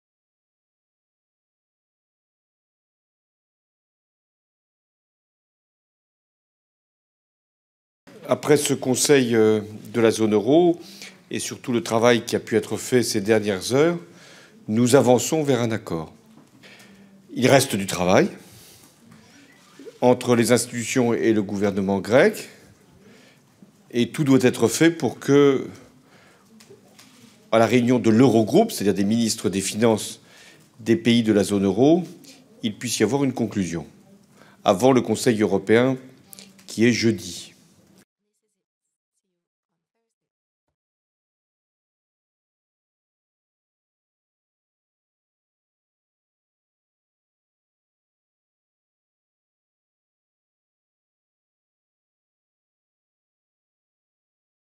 Hollande sur la Grèce: "nous avançons vers un accord"
Le président français François Hollande a estimé lundi 22 juin 2015 à Bruxelles, à l'issue d'un sommet de la zone euro, que la Grèce et ses créanciers avançaient "vers un accord" qu'il appelle de ses voeux cette semaine. Durée: 00:55